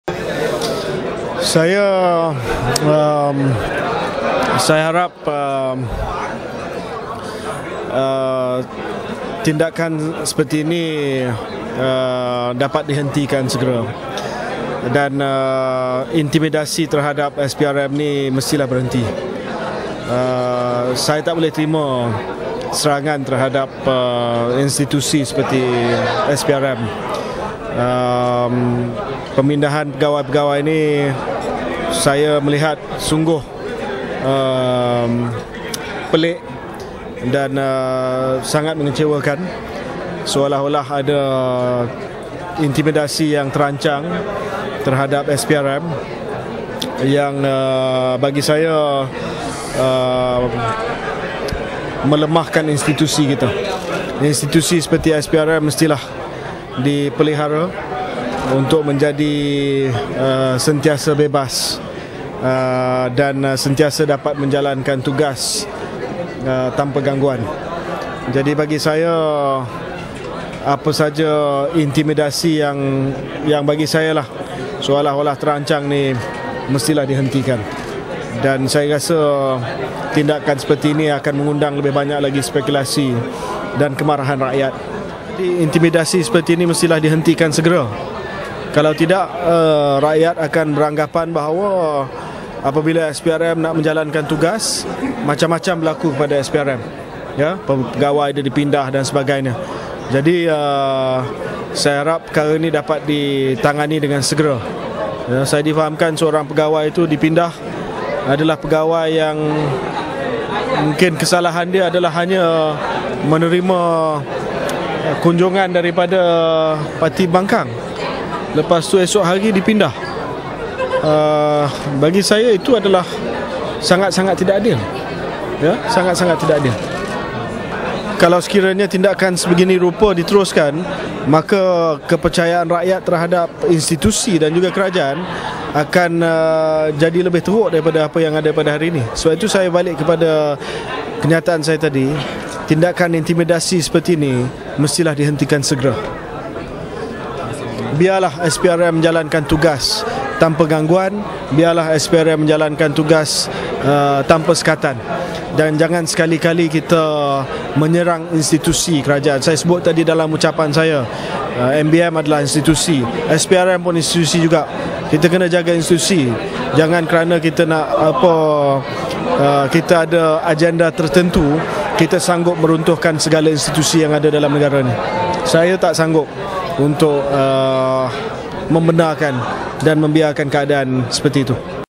"Intimidasi terhadap SPRM perlu dihentikan segera" - Khairy Jamaluddin
Respon Menteri Belia dan Sukan berkaitan pertukaran 2 pegawai kanan Suruhanjaya Pencegahan Rasuah Malaysia (SPRM) ke Jabatan Perdana Menteri. Bagi beliau, tindakan mengganggu tugas SPRM perlu dihentikan segera malah, tindakan seperti ini pada beliau amat pelik.